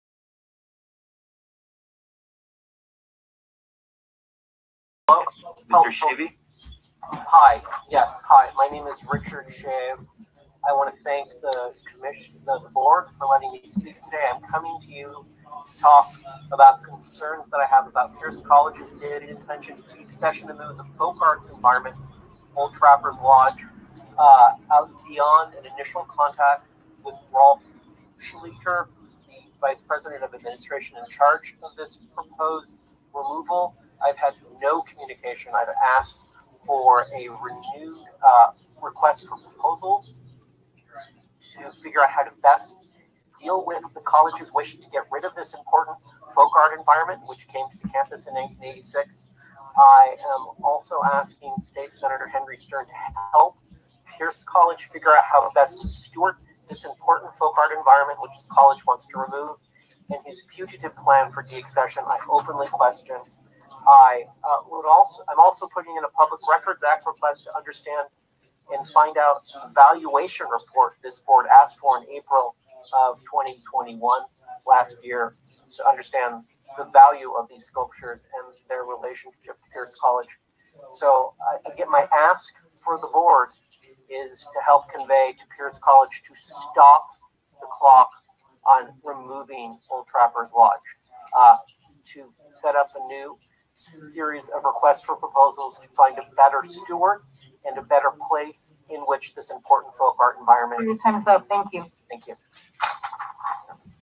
Richard Schave makes public comment to LACCD about threatened Old Trapper's Lodge (4/13/2022)
In a remote section of the campus of Pierce College in the San Fernando Valley, there is a remarkable and little known folk art environment, Old Trapper’s Lodge (California State Landmark 939.5)—but it might not be there for much longer.

If the artwork must be moved from campus, we are advocating that an RFP (request for proposals) be circulated to find the best possible steward for the landmark.

On April 13, 2022 our Richard Schave again made public comment to the LACCD board about his concerns about Pierce College's lack of transparency regarding their stated intent to deaccession Old Trapper's Lodge, the college administration ignoring repeated inquiries and locking the public out so nobody can visit the sculptures.